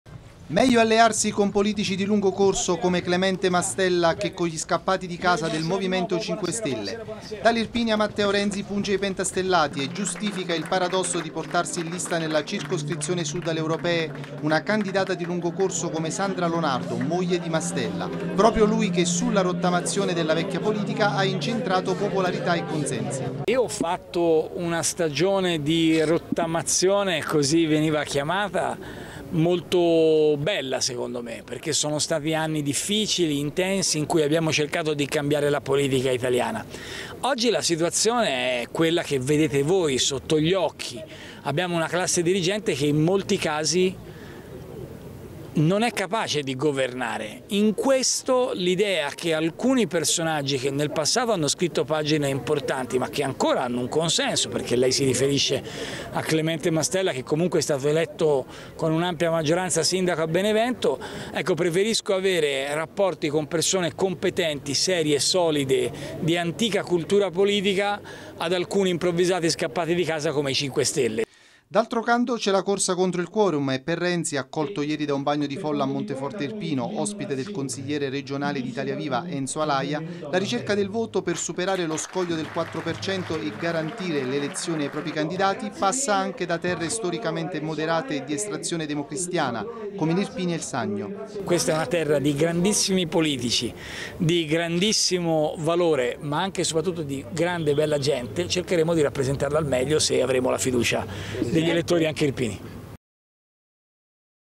Assist di Renzi a Mastella e De Luca: “Li preferisco agli scappati di casa”
Il leader di Italia Viva a Monteforte spiega l’alleanza con la Bonino alla Europee. Difende Sandra Mastella e, sull’autonomia differenziata, il governatore De Luca. “Meglio loro che gli scappati di casa dei 5 Stelle”.